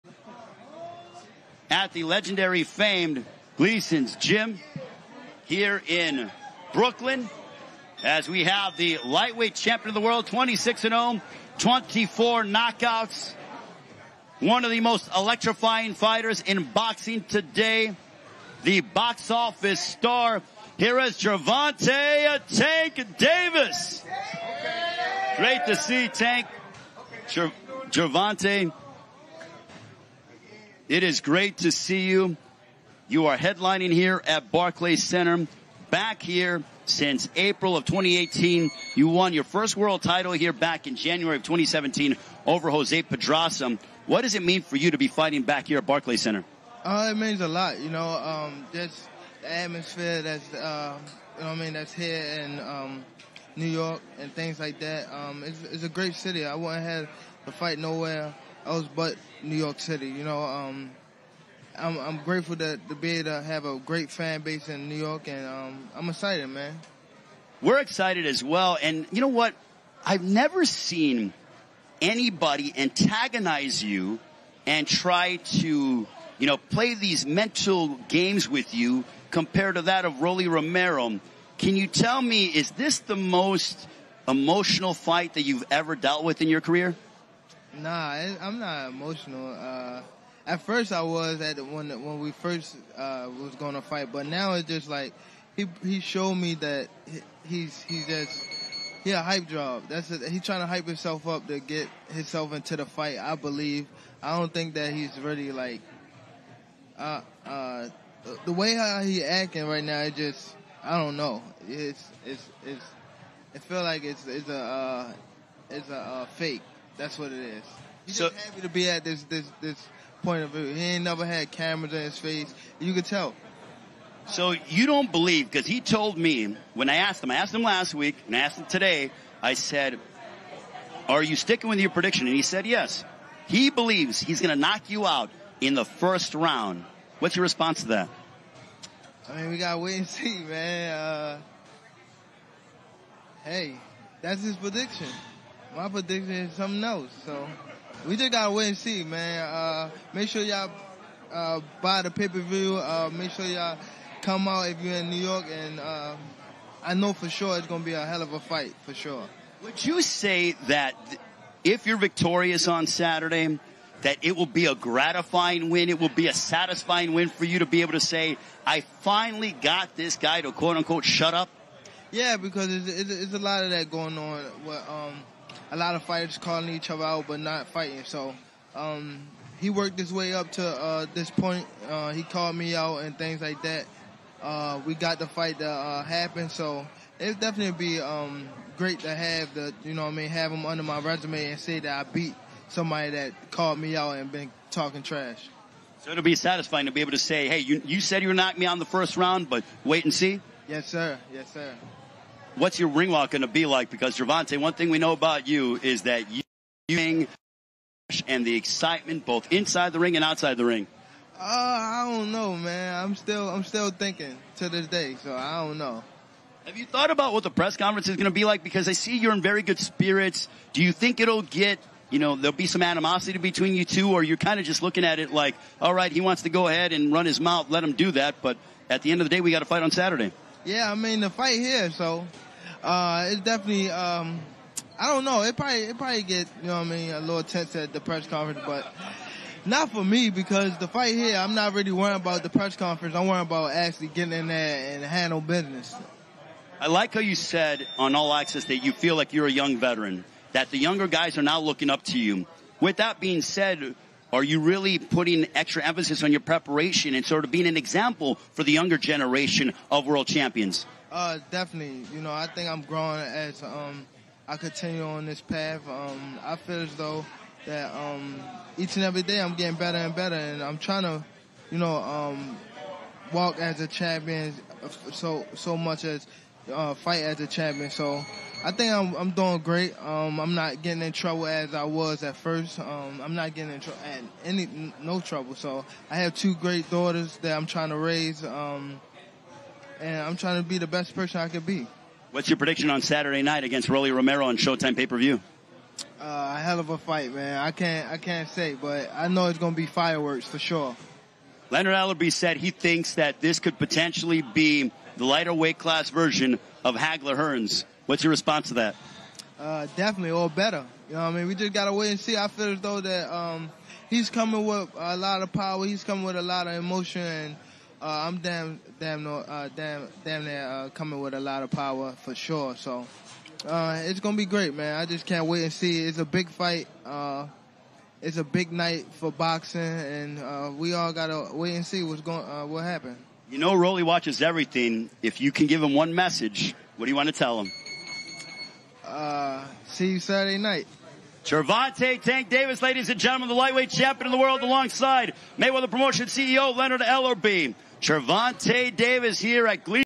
Davis vs Romero | Gervonta Davis' message to "Rollie" Romero before Saturday's fight
Gervonta Davis talks with Ray Flores at his final media workout ahead of his bout with Rolando "Rollie" Romero on Saturday, May 28, on Showtime.